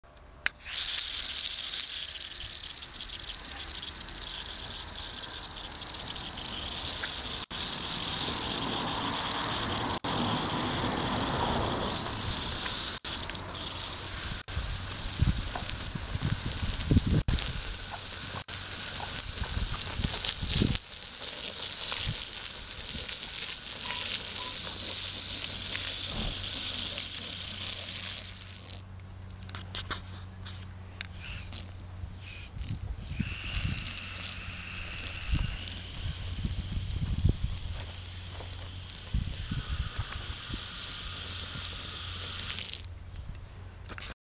Wasserratte
Das Monster beim Frühsport...es kann so niedlich sein! ;-)